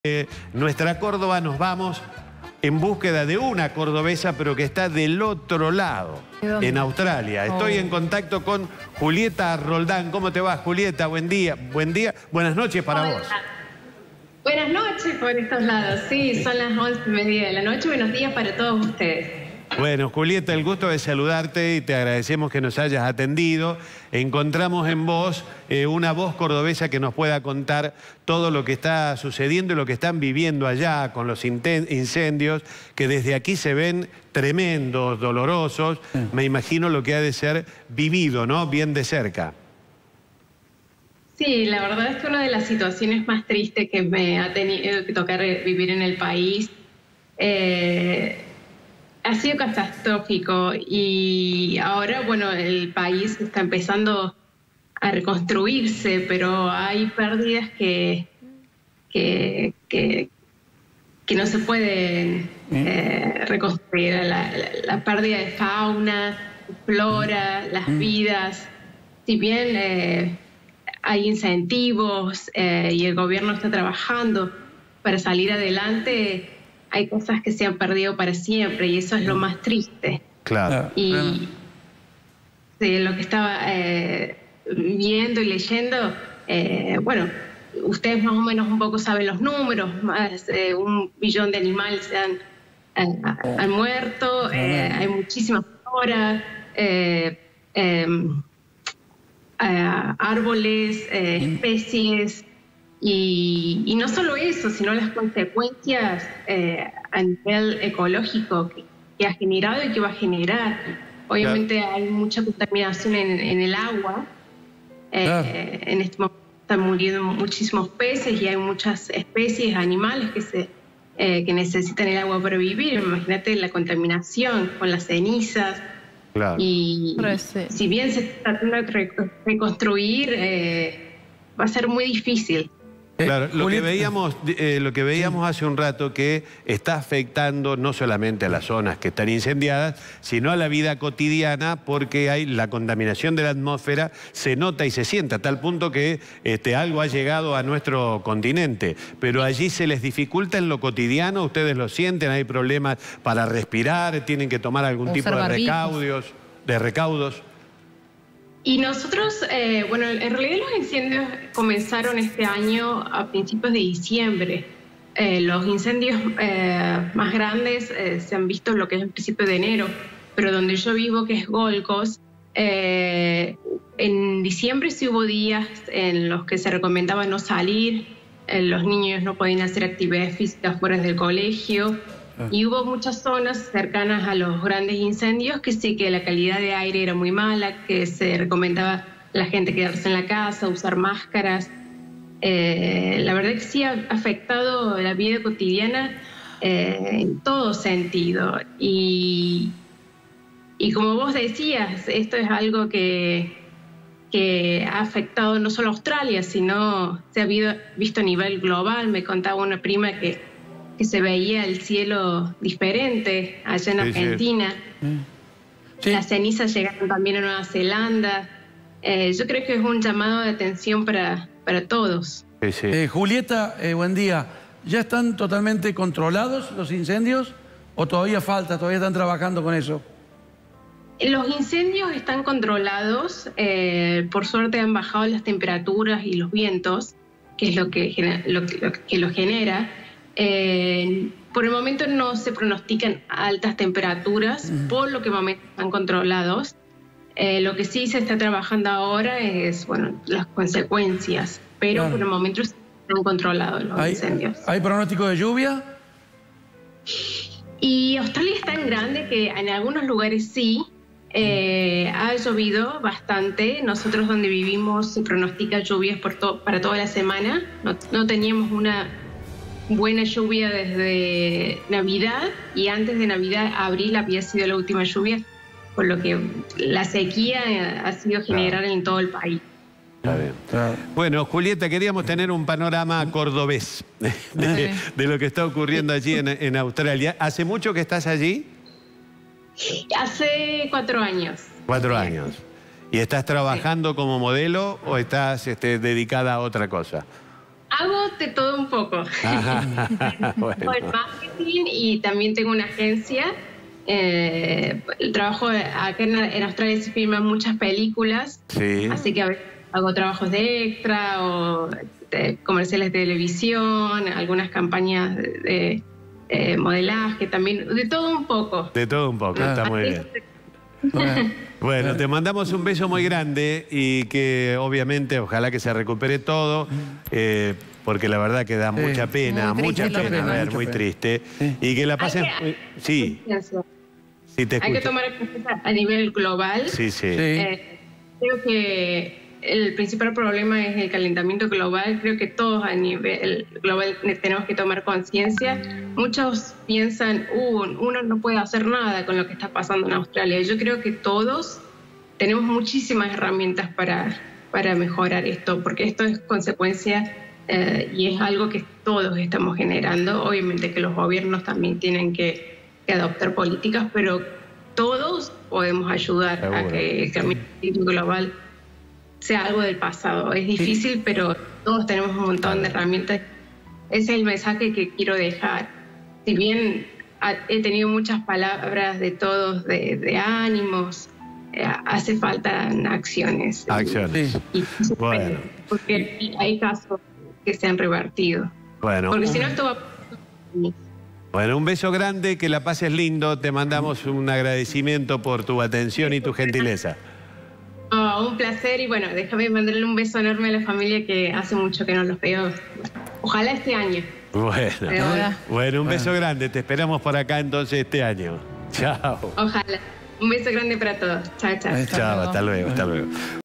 Los incendios de Australia contados por una cordobesa
Julieta Roldán es una estudiante cordobesa que actualmente reside en Australia, y que vivió de cerca los incendios que devastaron al país. Por medio de comunicación vía Skype, Julieta le contó al equipo del Show cómo fue su experiencia en el país oceánico.